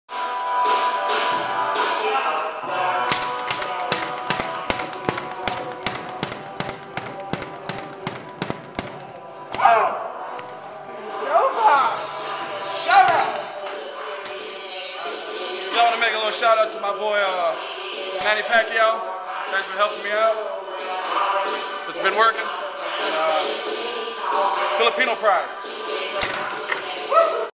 Rey Maualuga boxing shoutout to Manny Pacquiao
During his work on the speed bag, Rey gave a shout out to Manny Pacquiao.